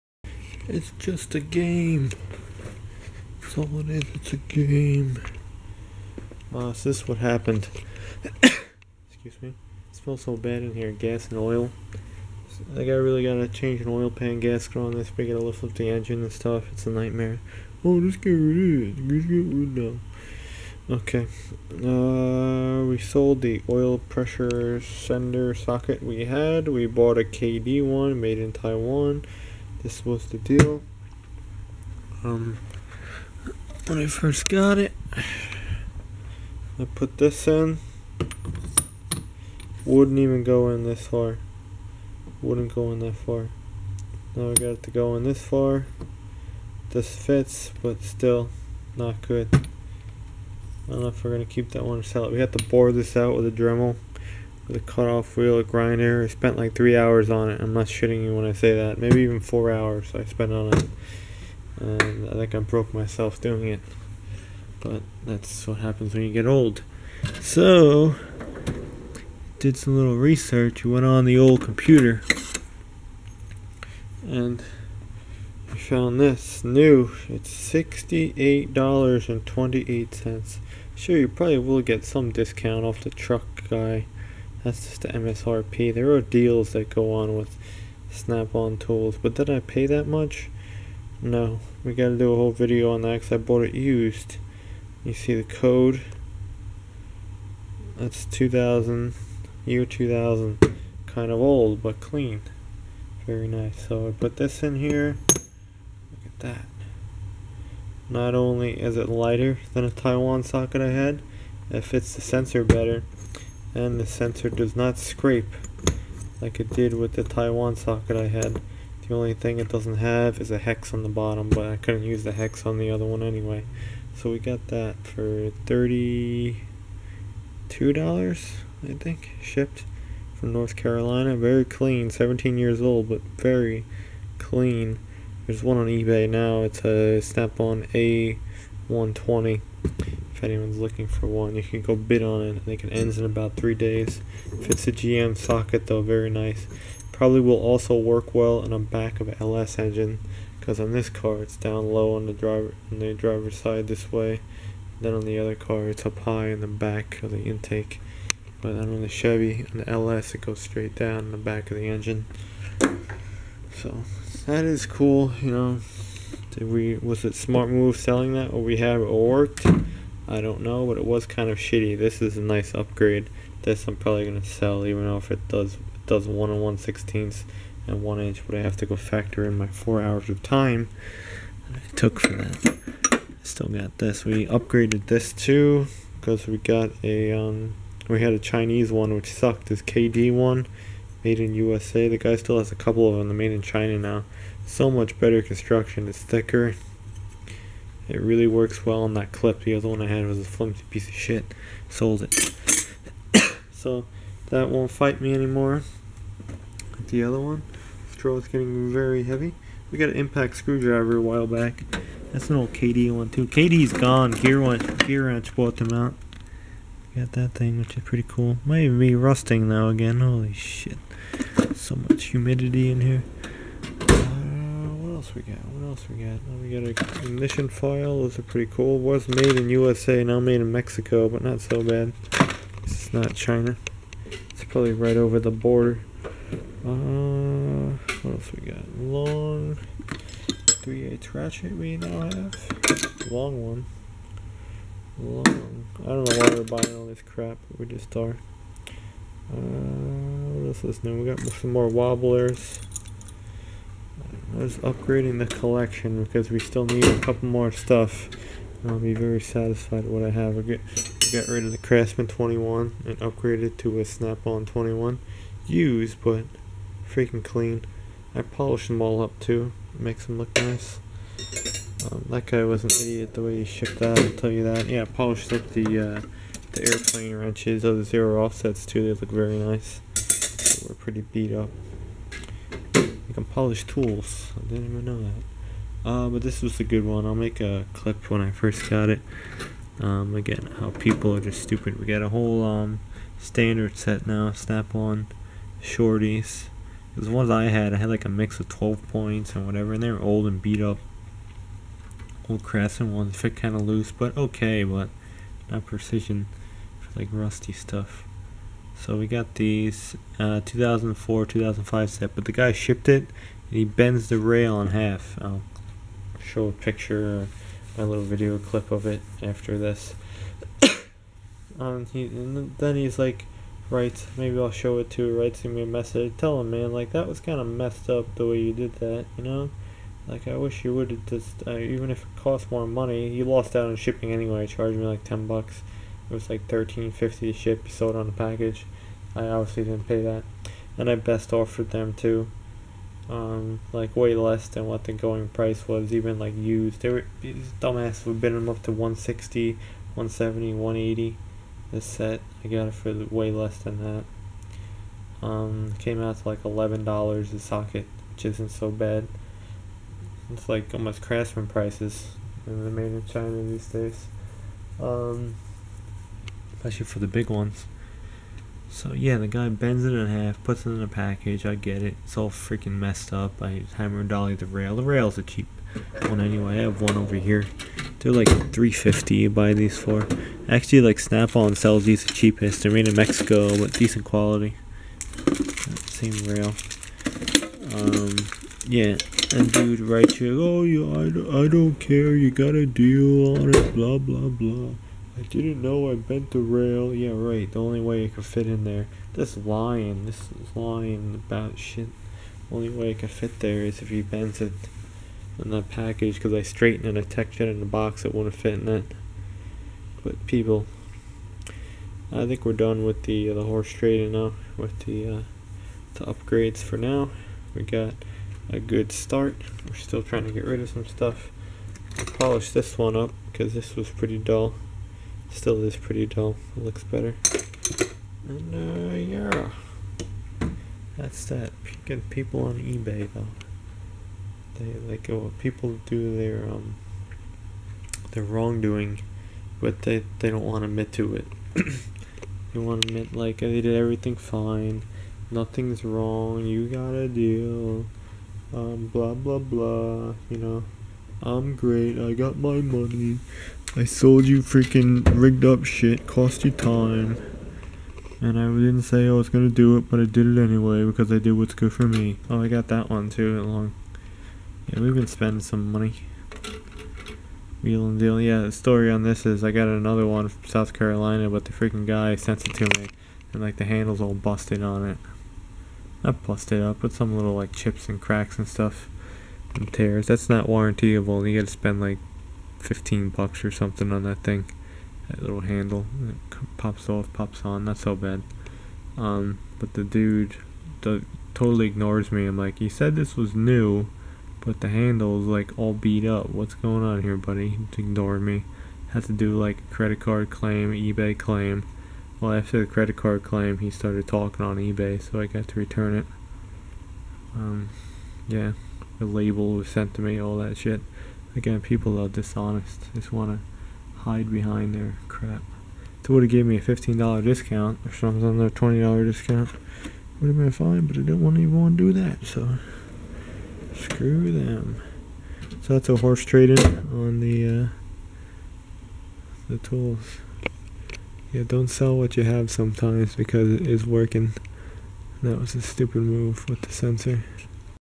Sold what worked,, what an idiot, But ghetto upgraded!
Snap off sensor socket rules. A120, get one if you need one it works
All I do for now is play with tools. too hot to play cars or junkyard games